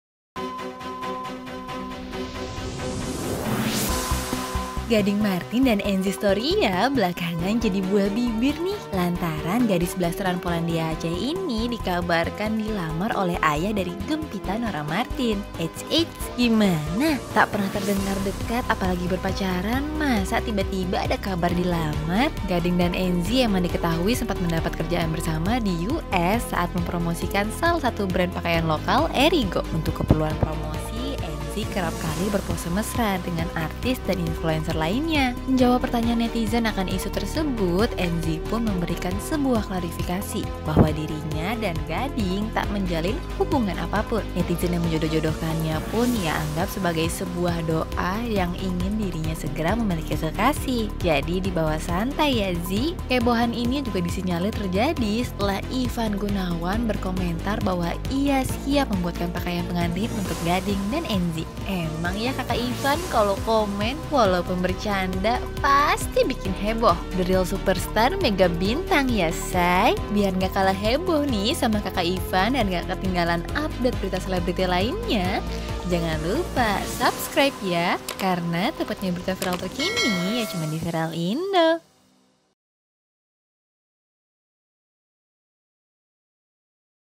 Liburan Mesra Bersama Enzy Storia, Gading Marten Lepas Status Duda?
Gading Marten dikabarkan dekat dengan Enzy Storia!
Setelah diisukan dekat dengan Omar Daniel, Gading beberapa kali mengunggah kedekatannya dengan Enzy Storia di Instagram.

Hal ini tentu membuat para artis lainnya ramai-ramai komen di sosial medianya.
Bahkan Ivan Gunawan komentar siap membuatkan baju nikah untuk Enzy jika menikah nanti.
Apakah benar Gading dan Enzy berhubungan dekat?

Nah kali ini minal akan bahas hubungan Enzy dan Gading

Nyalakan juga loncengnya biar keep uptodate video Viral selanjutnya!

Viral Indo!
Berita Viral Terkini ya cuma disini!